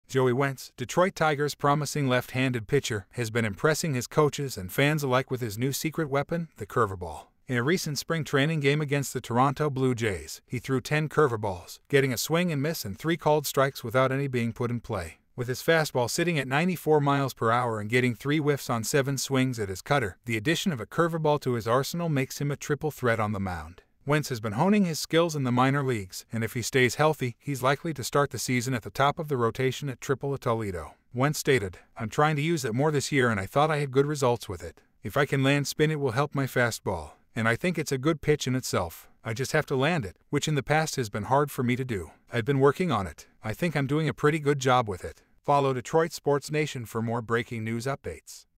Detroit Tigers P Joey Wentz busts out secret weapon for 2023
Welcome to Detroit Sports Nation News, your source for the latest updates on Detroit Tigers' promising left-handed pitcher Joey Wentz. Discover Wentz's new secret weapon - the curveball - and how it's making him a triple threat on the mound. Watch as we explore his impressive performance in the recent Spring Training game against the Toronto Blue Jays and discuss his future in the minor leagues.